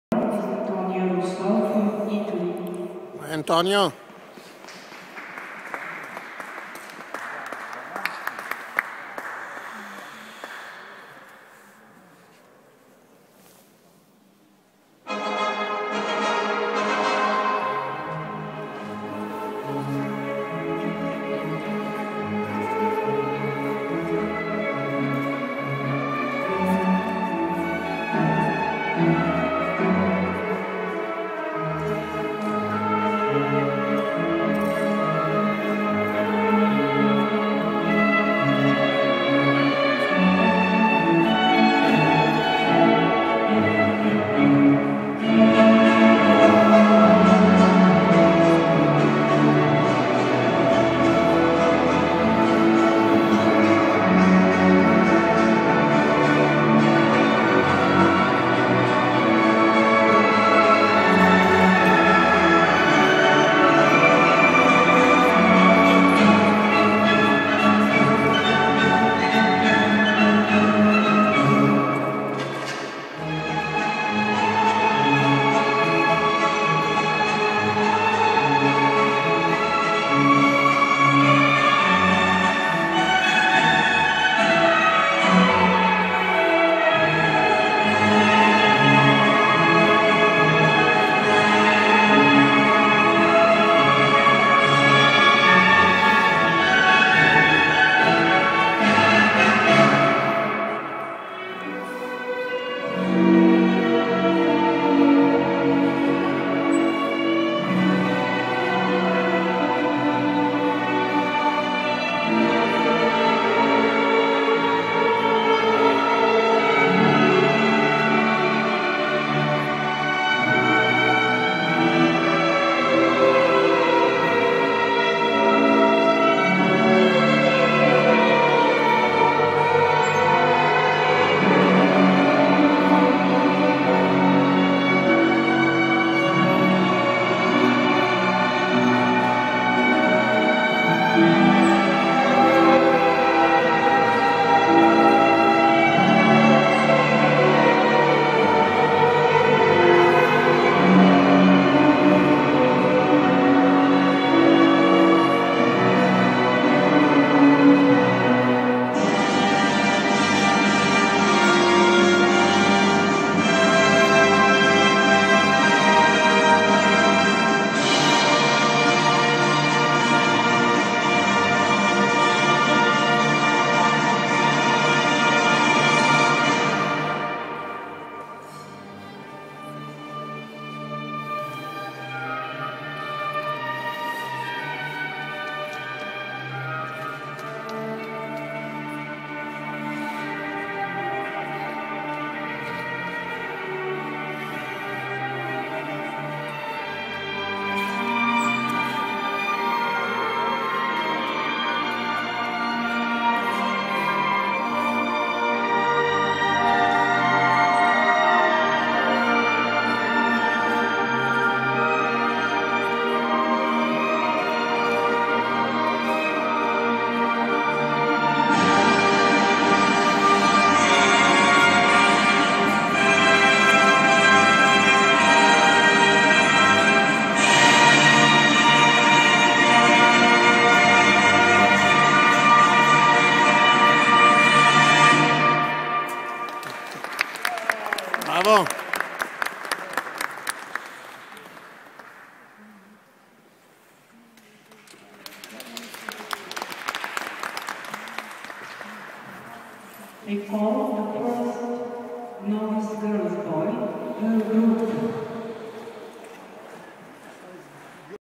Antonio Russo - Primo classificato categoria Advanced Novice Boys V COPPA EUROPA SKATE HELENA
Primo classificato nella categoria advanced novice Boys a Belgrado nella gara SKATE HELENA.